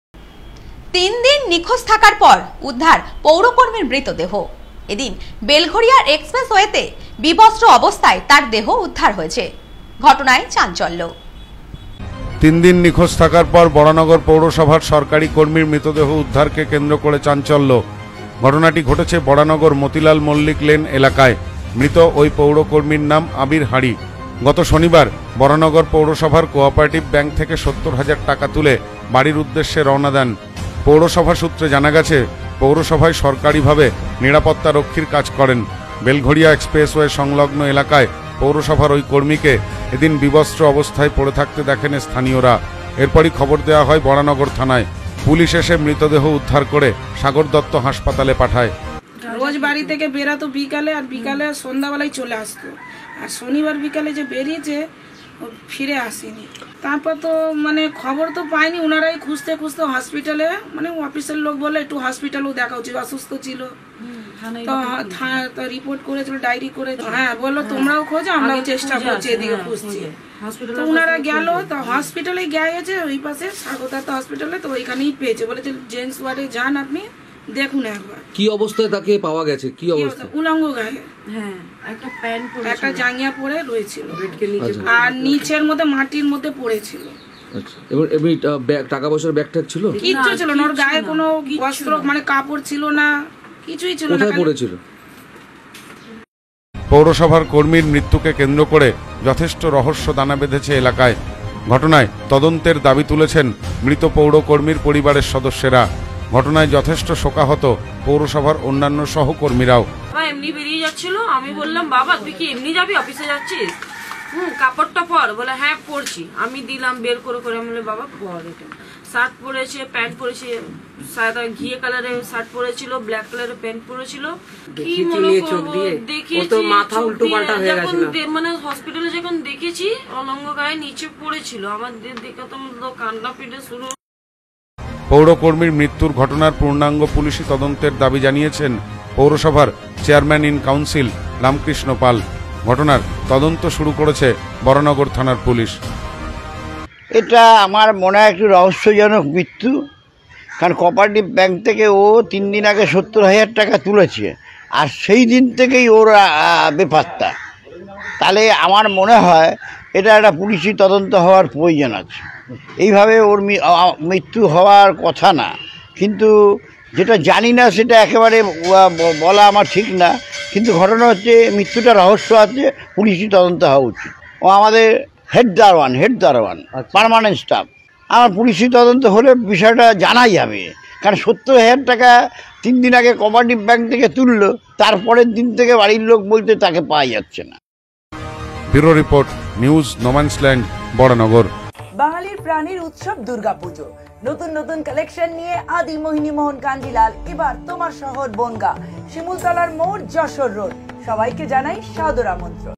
৩ দিন নিখোঁজ থাকার পর উদ্ধার পৌর কর্মীর মৃতদেহ ৷
৩ দিন নিখোঁজ থাকার পর উদ্ধার পৌর কর্মীর মৃতদেহ ৷এদিন বেলঘড়িয়া এক্সপ্রেস ওয়েতে বিবস্ত্র অবস্থায় ওই কর্মীর দেহ উদ্ধার হয় ৷ঘটনায় চাঞ্চল্য ৷